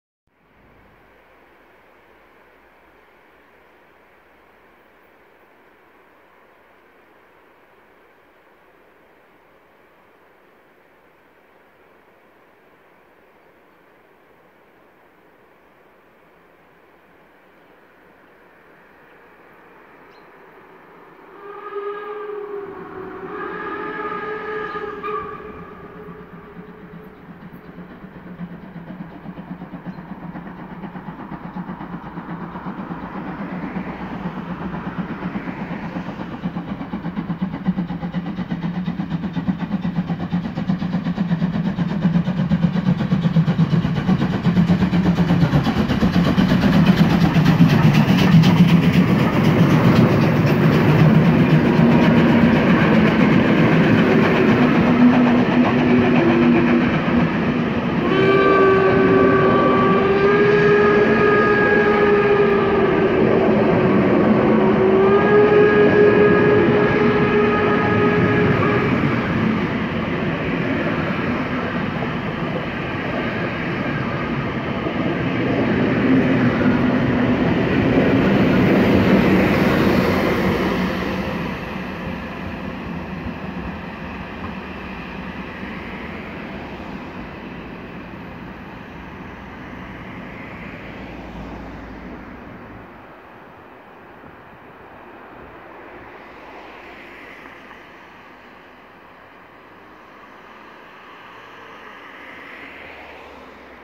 35 1097 in Melsungen - Bartenwetzerbrücke
Auf der Fahrt zum Erfurter Weihnachtsmarkt, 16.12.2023